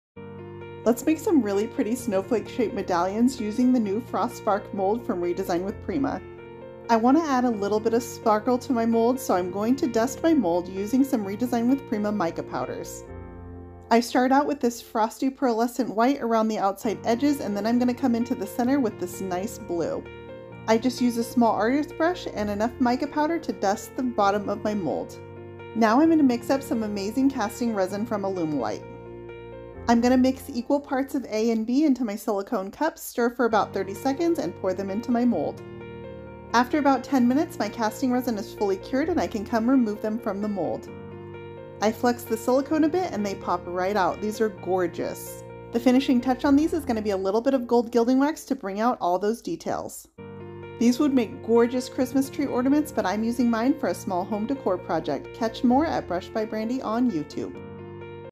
The new Frost spark mould from Redesign with Prima makes for beautiful holiday decorations
The new Frost spark mould from Redesign with Prima makes for beautiful holiday decorations, see what I made at the end!